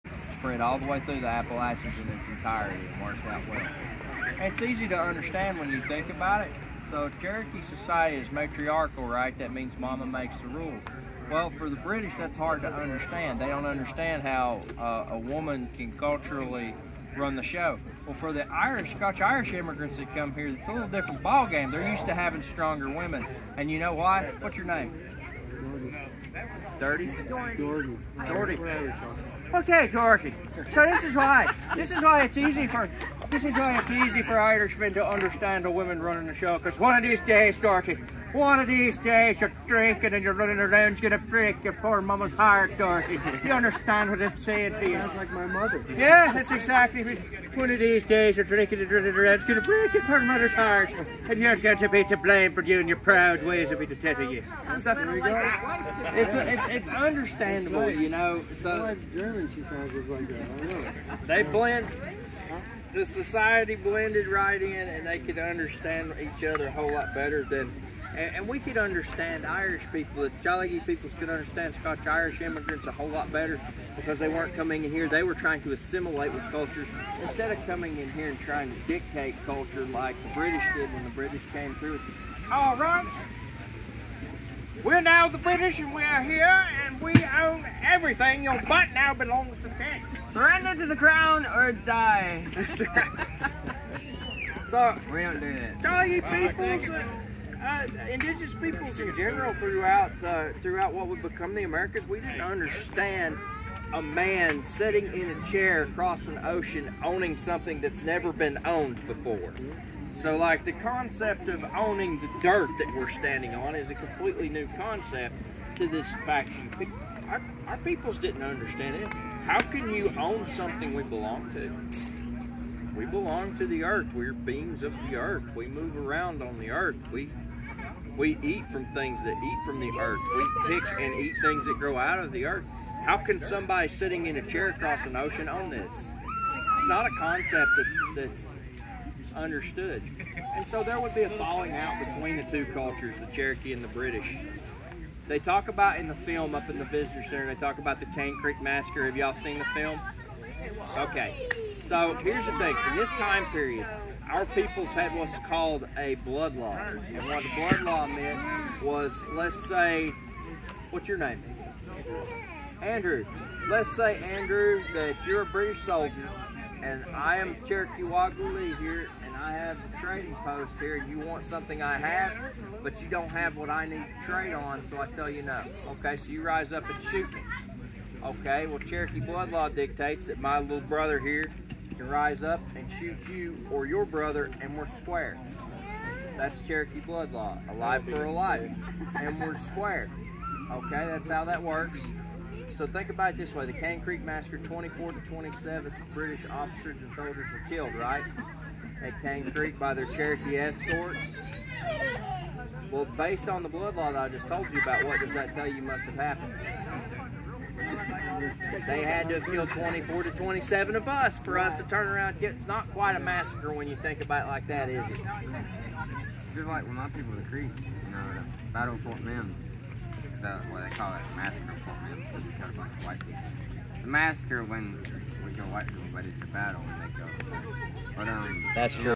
The Clash of Cultures-shared at Fort Loudoun Garrison Weekend reenactment
The reenactors at the Fort Loudon Garrison Weekend were absolutely amazing! They interacted with the crowd, and each one I spoke with was incredibly friendly. Their stories captivated me and the crowd, leaving us all eager for more. Please listen to a video clip of one of those talented story tellers as he weaves the history of the Cherokee matriarchy, blood law and belonging to the land!  J. Earl Baldwin